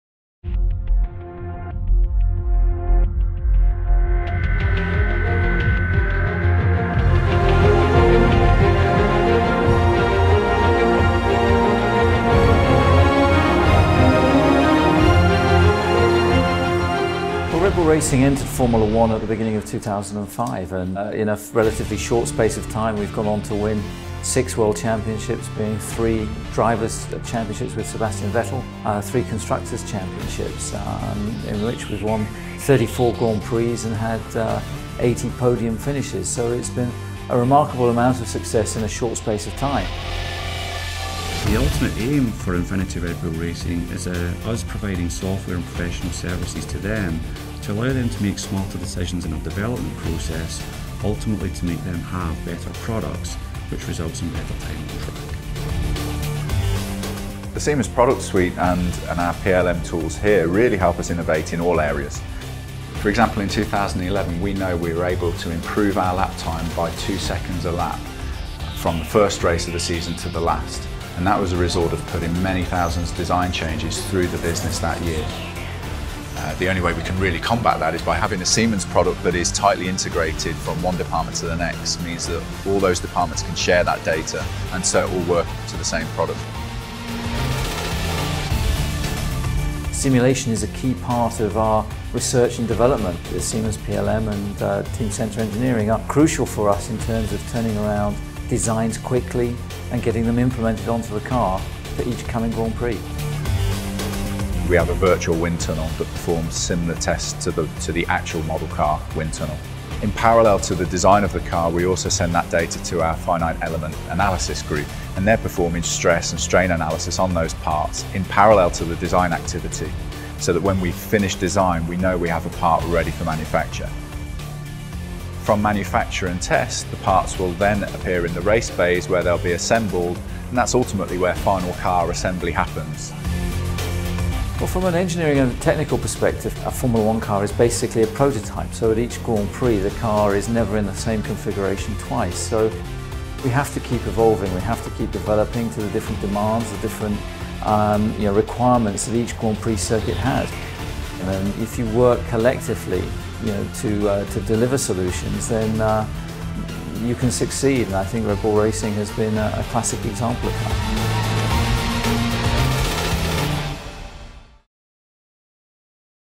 Innovation never sleeps - a F1 car between races
What happens between Formula One races? How are the Formula One Cars adjusted to each GP race track? See how Infiniti Red Bull Racing takes advantage of Teamcenter and NX solutions from Siemens PLM to ensure continuous development of the RB9 and improve on-track performance on a race-to-race basis. Siemens PLM is the Innovation Partner of Infiniti Red Bull Racing.